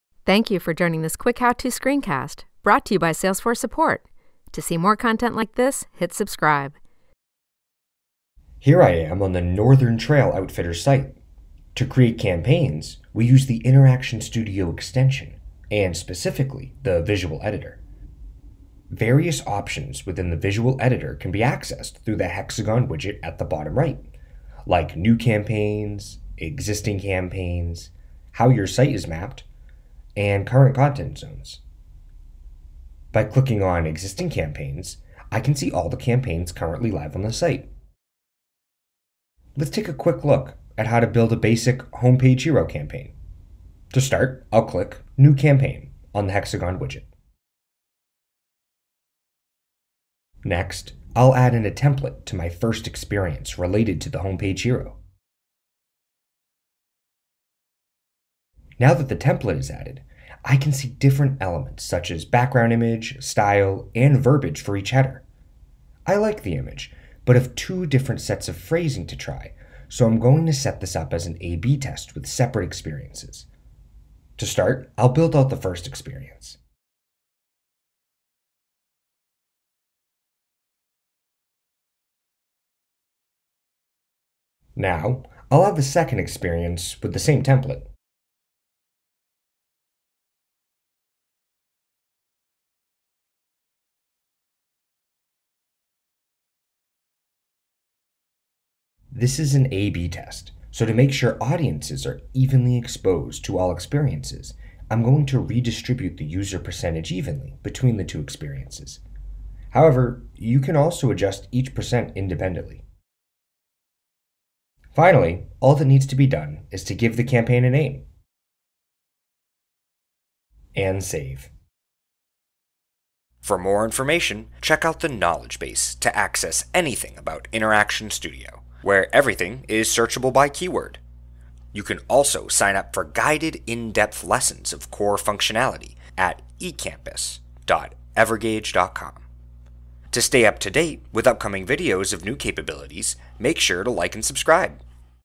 How to Create A/B Test and Control Groups in Marketing Cloud Personalization
After watching this screencast the user will be able to: Launch the visual editor to view new campaigns, existing campaigns, current content zones, and how your site is mapped to the Interaction Studio UI. Build a A/B test campaign. Insert a template to two different experiences. Add a control hold out for reviewing campaign results. Name and save your campaign.

Note: Marketing Cloud Personalization is the former Interaction Studio